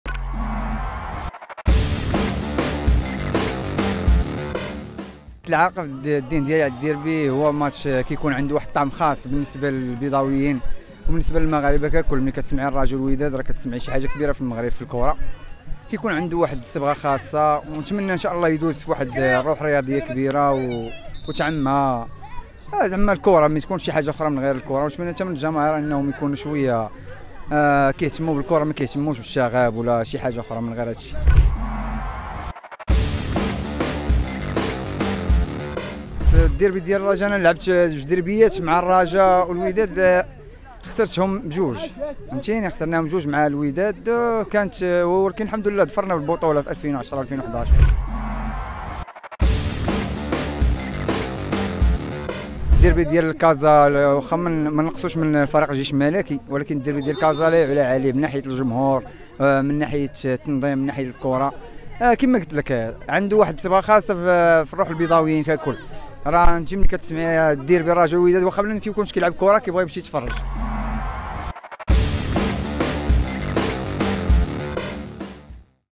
اللاعب ألاص: الديربي يسكن أرواح البيضاويين
أنفاس سبور تسلط الضوء على مباراة الديربي البيضاوي من خلال شهادات مجموعة من اللاعبين